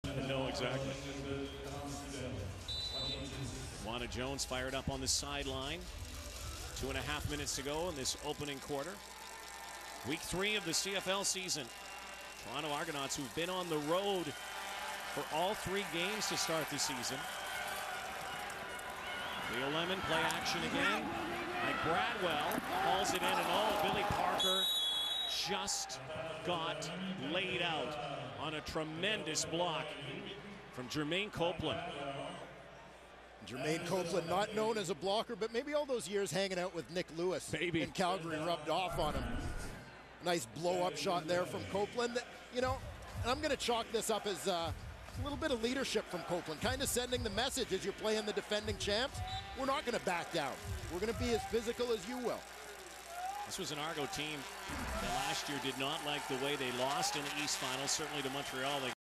Big block by Jeremaine Copeland
"Cope" throws all that he's got into this block to free up a few yards for his fellow receiver Mike Bradwell